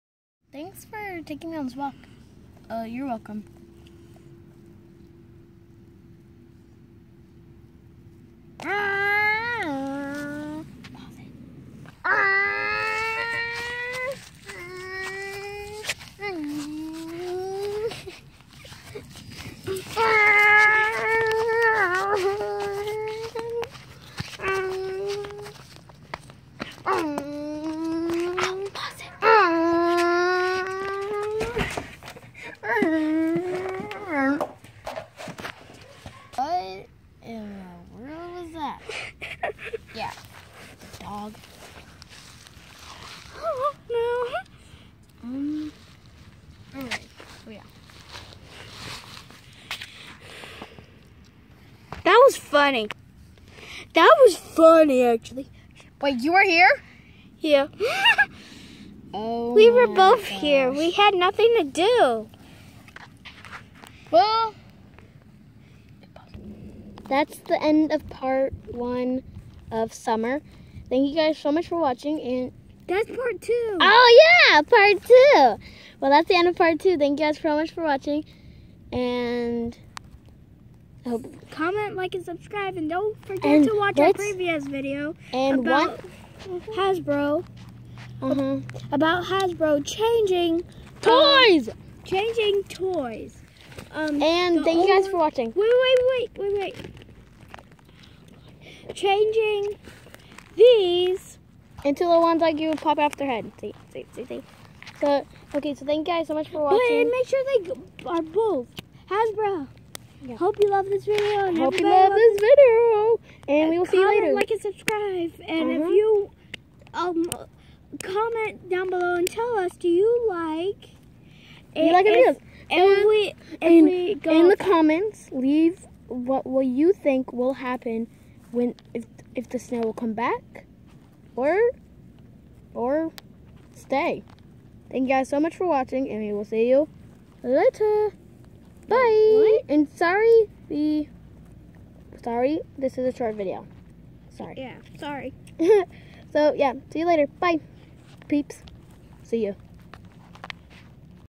LPS what the snail! Part 2 of summer
Just then in a good moment the snail has to ruin everything : [